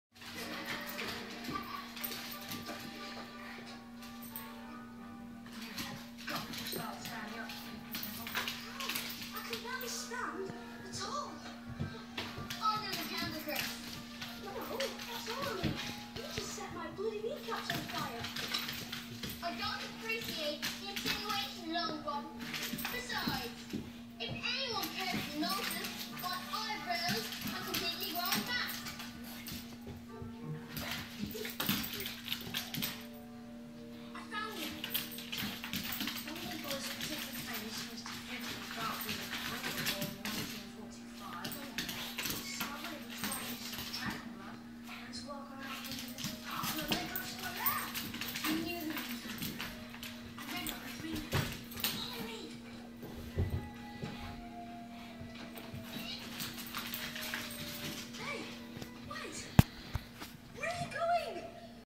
Nick and Murray afternoon melee. Deathmatch.
Two rescued animals go at it in afternoon melee.